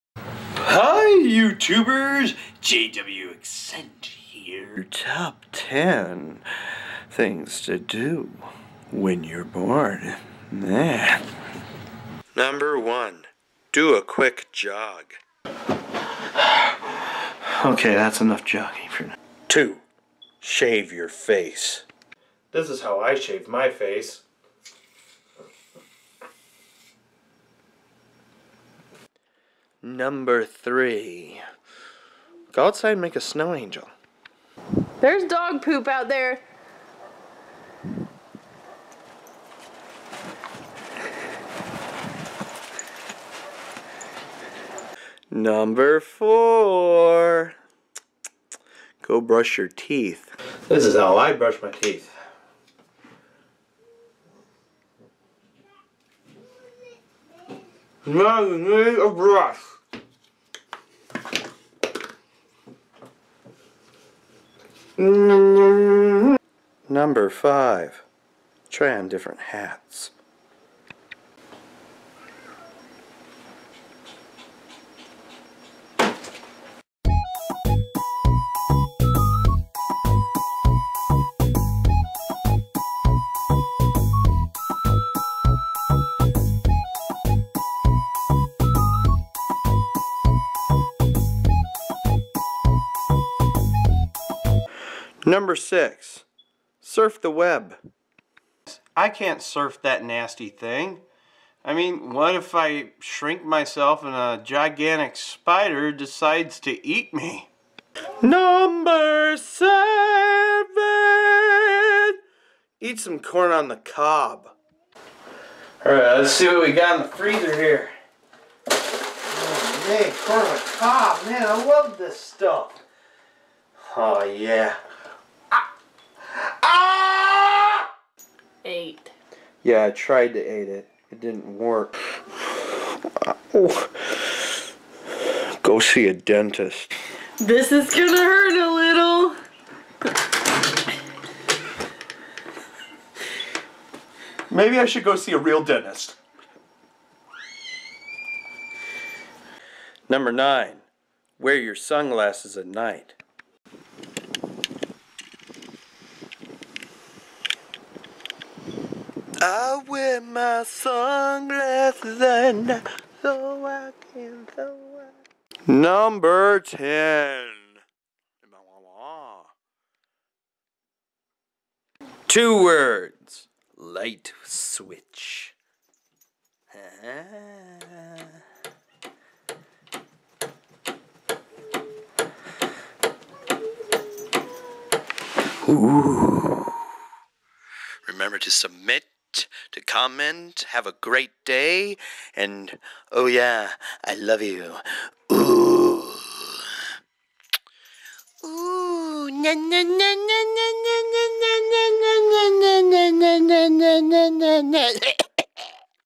10 Things To Do When Your Bored
We all get bored right? So here is my first ever 'Ten Things To Do When Your Bored' video of 10 things to do.. When you're bored.. Hope you enjoy!